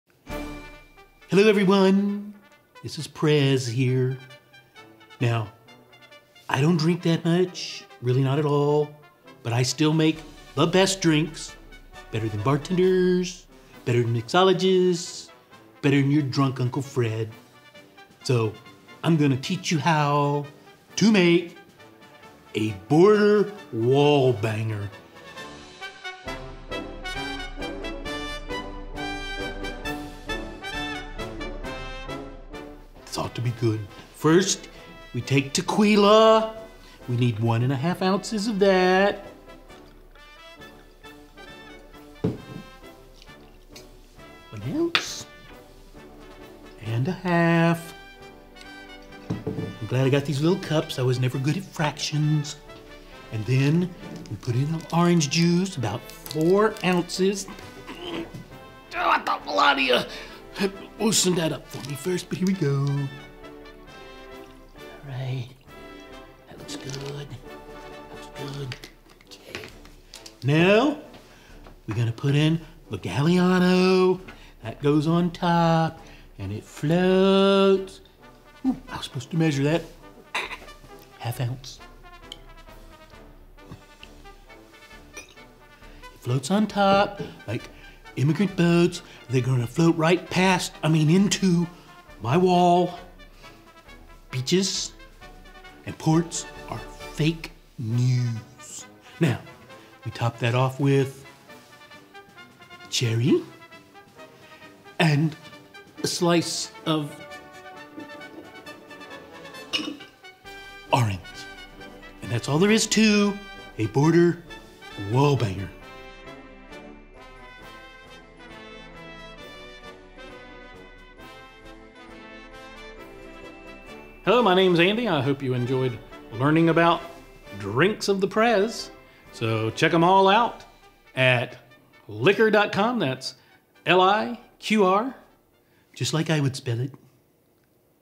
How to Make a Border Wallbanger with The Prez!
We have a State of Emergency here, folks! It's President's Day and we're honored to have The Prez with us today. Here he is making the Border Wallbanger.  Fake news!

Border Wallbanger Recipe
1 1/2 ounces Tequila
4 ounces Orange Juice
1/2 ounce Galliano

Fill glass with ice. Pour in tequila, orange juice, and Galliano.  Top with a cherry and an orange wedge.